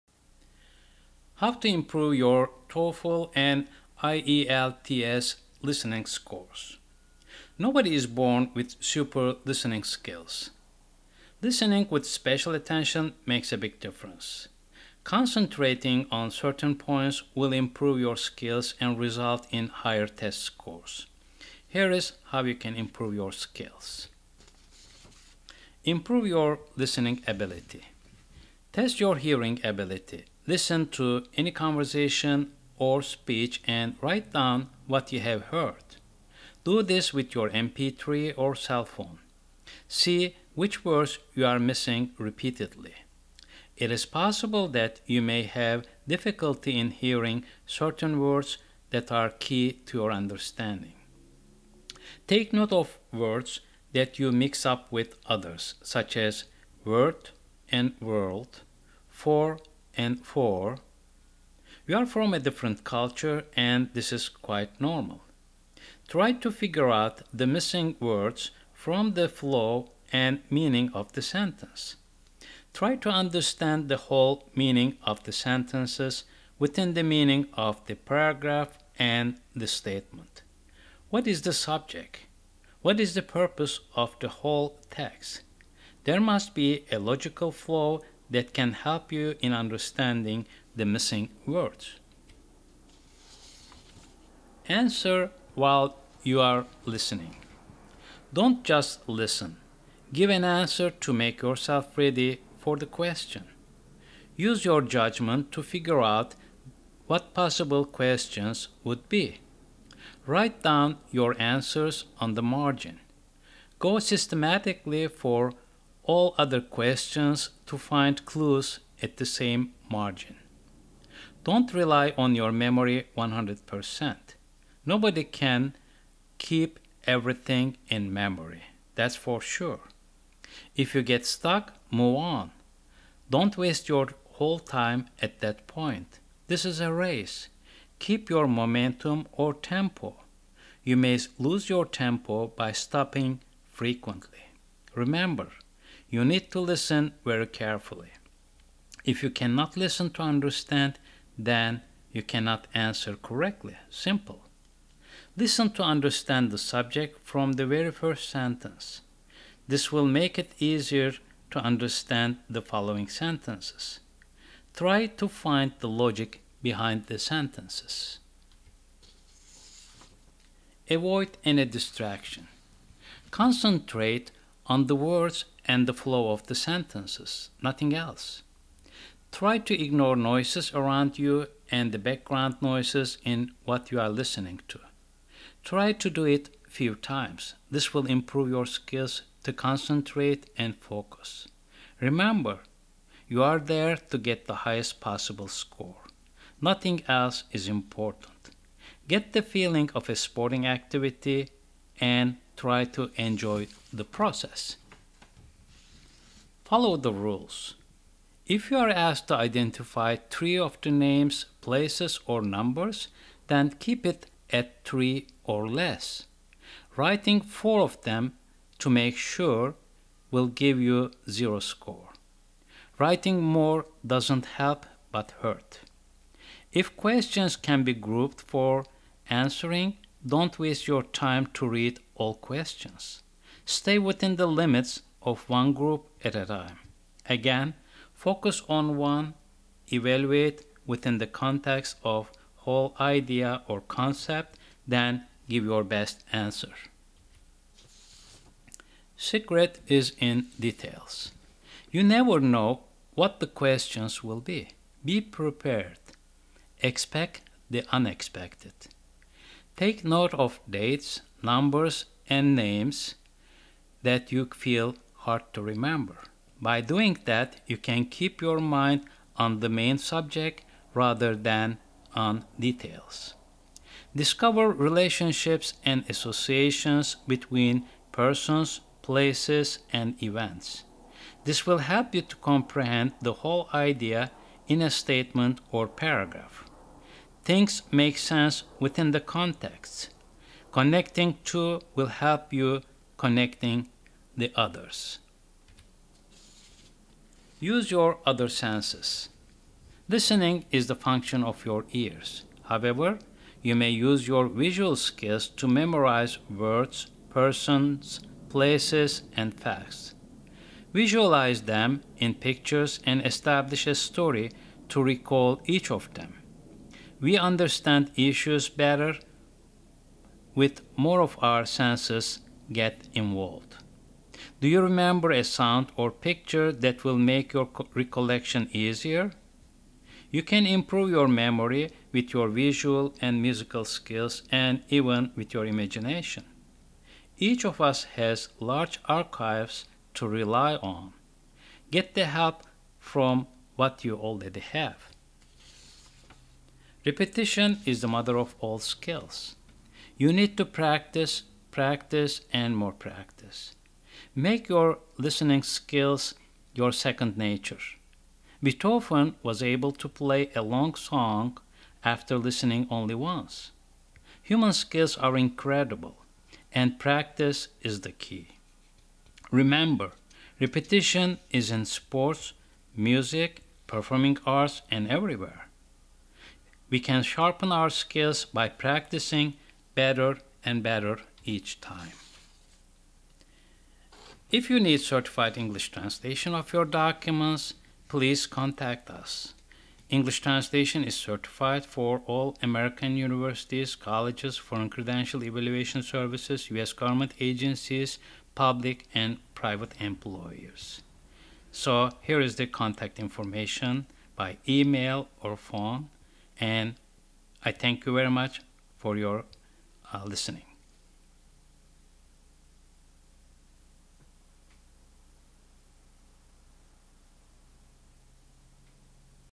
How to Improve your TOEFL and IELTS Scores?
Improve your listening skills in answering TOEFL and IELTS questions and get higher scores.
Learn from international students who have been accepted by American universities and colleges to study in the United States.
Simple methods, improved concentration and focus, keeping purpose of text/statement to discover meaning words that you may miss.